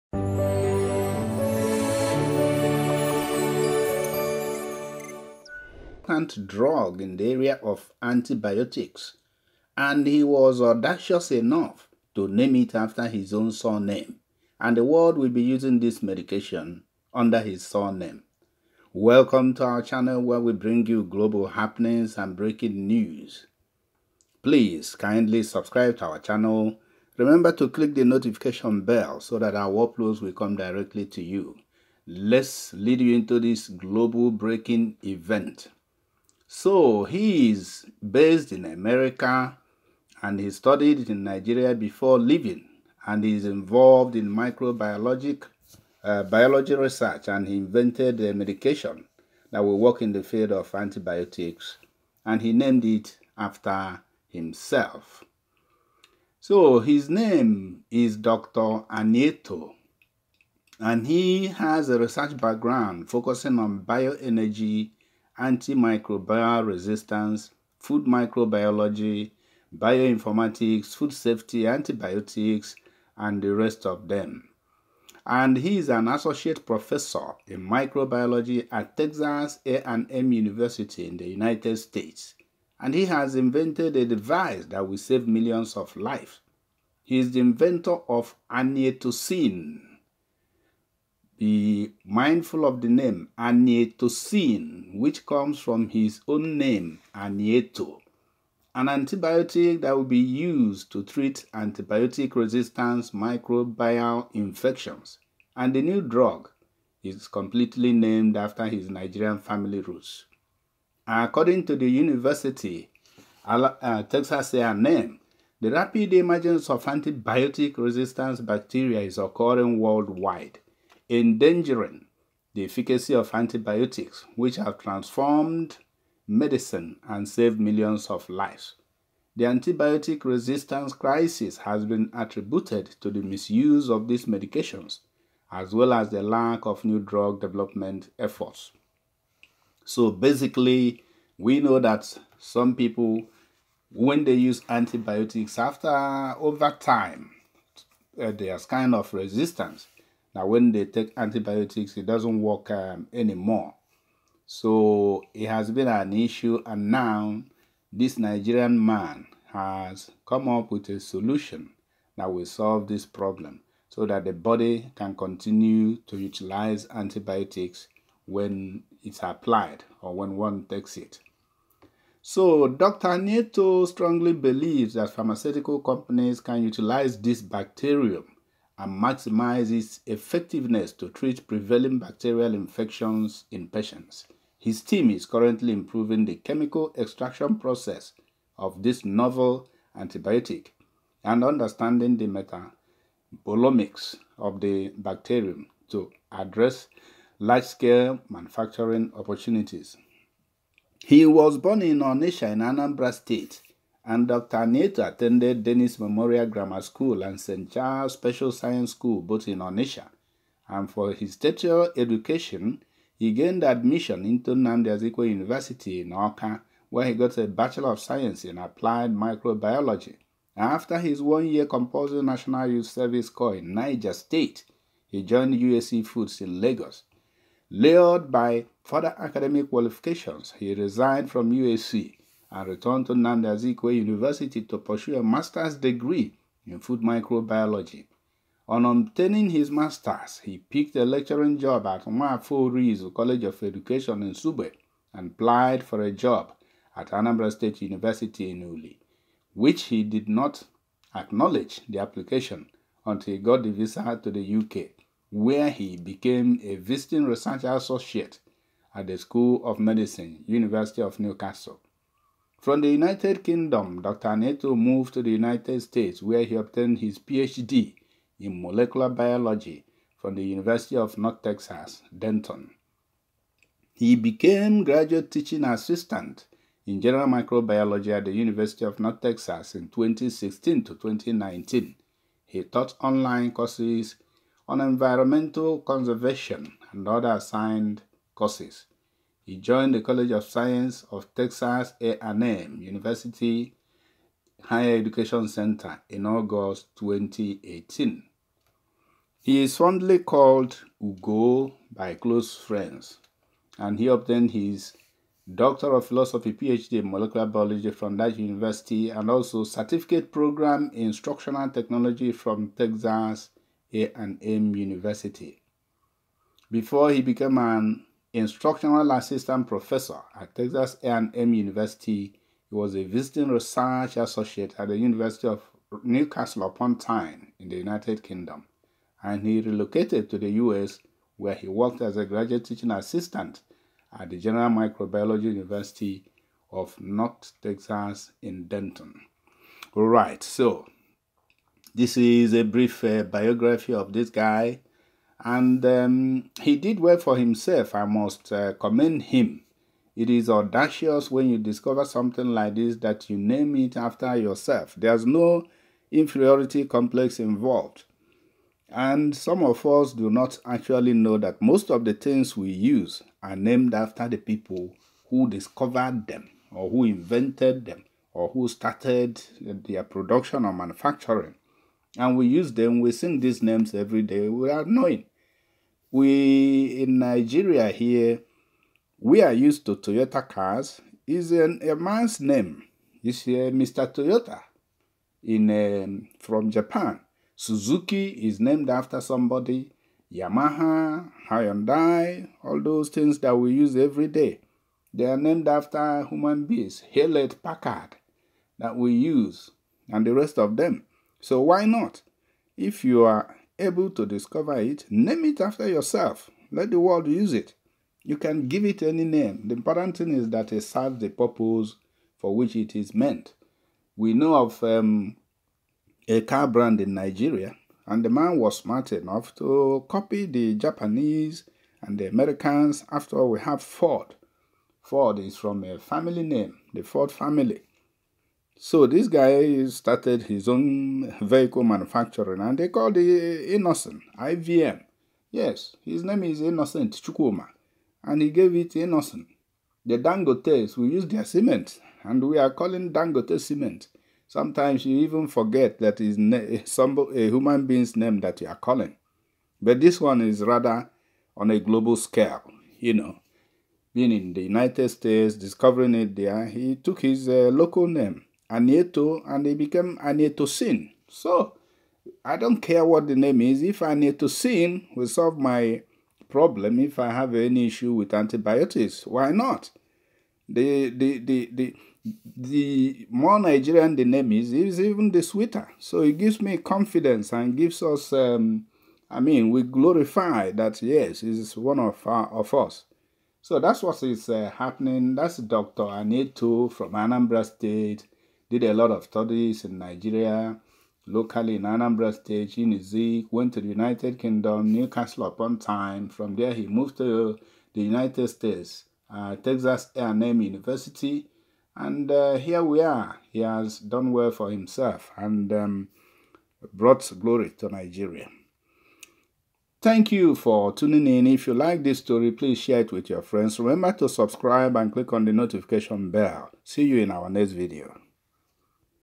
NIGERIAN GENIUS SHOCK THE WORLD WITH HIS DISCOVERY AND THE NAME WILL GLADDEN YOU
Anieto breaks bounds with new antibiotics resistance invention

NOT many inventions or discoveries see the light of day, more so becoming a global impact but Ugochukwu Obiakornobi Anieto has registered his name in the revered register of world changers.

Fate interfaced with a dream has a peculiar mode of turning the human path to global reckoning. Not many people set out reckoning an achievement transcends mere academic qualifications in a world that are in dire need of solutions to global challenges. Anieto, an Anambra State-born microbiologist belongs to that privileged class.

  The Anambra State-born food and environmental microbiologist with a research background focused on bioenergy, antimicrobial resistance, food microbiology, bioinformatics food safety, antibiotics, bioremediation and bioaugmentation, pharmaceutical and quality control microbiology has made a new foray into the area of drug invention with a ground-breaking discovery that will save millions of lives.

  Dr. Anieto, an Associate Professor in Microbiology at Texas A&M University, United States, has invented a device that will save millions of lives. He is the inventor of Anietocin, an antibiotic used to treat antibiotic-resistant microbial infections. The new drug is named after his Nigerian family roots.

  According to Texas A&M University, ”The rapid emergence of antibiotic-resistant bacteria is occurring worldwide, endangering the efficacy of antibiotics, which have transformed medicine and saved millions of lives. The antibiotic resistance crisis has been attributed to the misuse of these medications, as well as the lack of new drug development efforts. This disclosed invention presents a new strain of Pseudomonas. spp. bacteria that produce antimicrobial compounds effective against both Gram-positive and Gram-negative bacteria.”

  “A previously unknown Pseudomonas strain has been identified and now named “Anieto”, after the TAMU inventor Dr. Anieto. The bacterium produces a novel antibiotic compound, named “Anietoci, and has been tested against several bacteria, both Gram-positive and Gram-negative, for growth inhibition and has so far been effective against all test bacterial species. While resistance to antibiotics is a growing global concern, this antibiotic offers the promise of alleviating the substantial health and economic burden on healthcare systems and the population.

  Dr. Anieto strongly believes that pharmaceutical companies can utilise this bacterium and maximise its effectiveness to treat prevailing bacterial infections in patients. His team is currently improving the chemical extraction process of this novel antibiotic and understanding the metabolomics of the bacterium to address. large scale manufacturing opportunities.”

  Born in Onitsha, Anambra State, Dr Anieto attended Dennis Memorial Grammar School and St Charles Special Science School, both in Onitsha. For his tertiary education. He gained admission into Nnamdi Azikiwe University, Awka, where he got a Bachelor of Science in Applied Microbiology.  After his one-year compulsory National Youth Service Corps in Niger State, he joined UAC Food in Lagos. Lured by further academic qualifications, he resigned from UAC and returned to Nnamdi Azikiwe University to pursue his Master’s Degree in Food Microbiology.

  On obtaining his Master’s, he picked a lecturing job at Nwafor Orizu College of Education, Nsugbe, and applied for a job at Anambra State University, Uli, which did not acknowledge the application until he got the visa to the U.K, where he became a visiting Research Associate at the School of Medicine, University of Newcastle.

  From the United Kingdom, Dr. Anieto moved to the United States, where he obtained his PhD in Molecular Biology from the University of North Texas, Denton.

  He became Graduate Teaching Assistant in General Microbiology at the University of North Texas in 2016 to 2019, he taught online courses on Environmental Conservation and other assigned courses. He joined the College of Science of Texas A&M University McAllen Higher Education Centre in August of 2018.